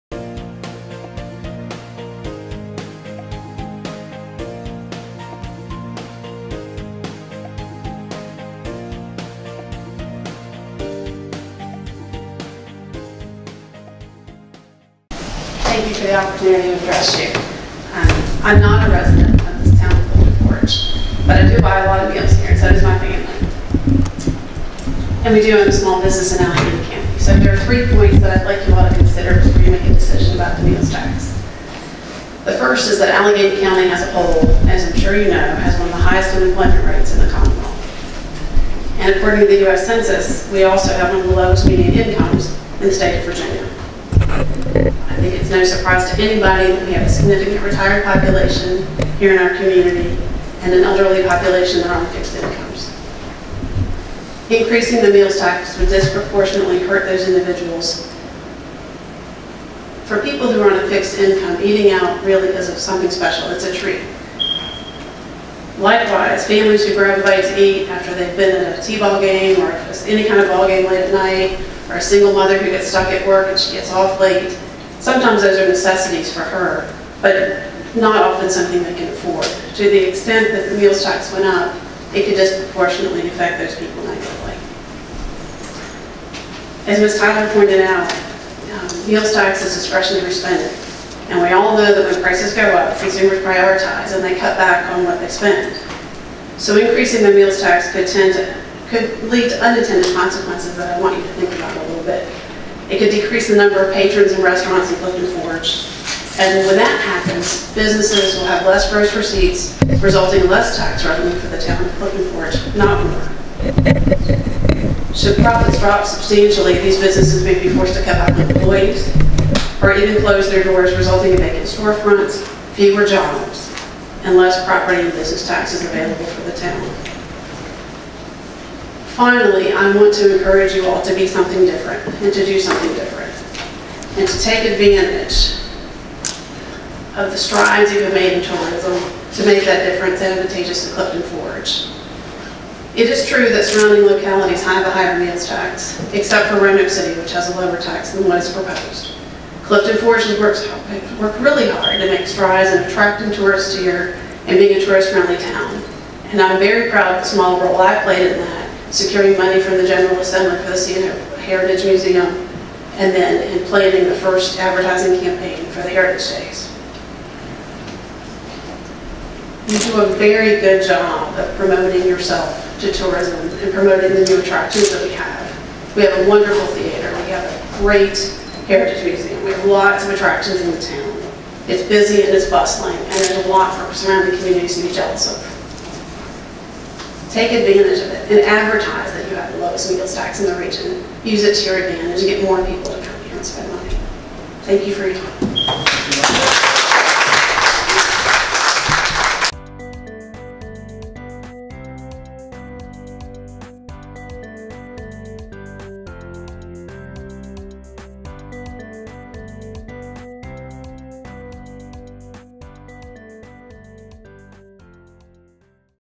Lethia Hammond Speaks To Clifton Forge Town Council On Meals Tax
Regional resident Lethia Hammond speaks to the Clifton Forge Town Council (on May 8, 2018) about her views on the Clifton Forge meals tax proposal.